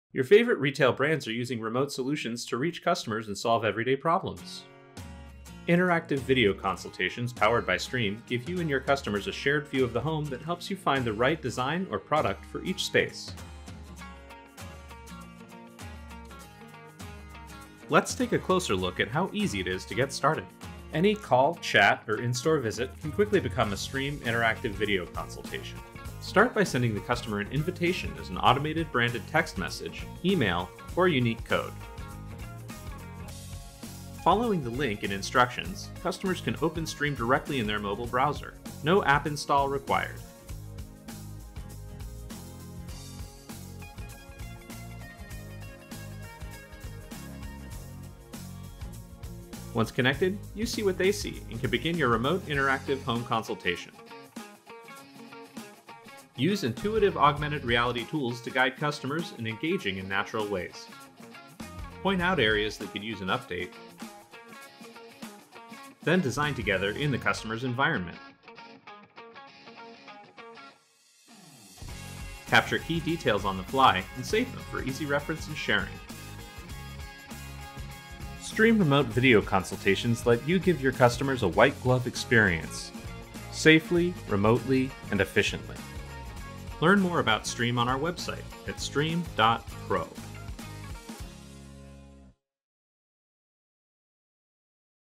Remote Retail Consultations | Streem® Visual Assistance
Your favorite retail brands are using remote visual assistance to reach customers and solve everyday problems. Interactive video consultations powered by Streem give you and your customers a shared view of the home that helps you find the right design or product for each space.

Interactive Video Consultations:

Connect with customers at the right time, in the right place, with the right details to provide excellent experiences.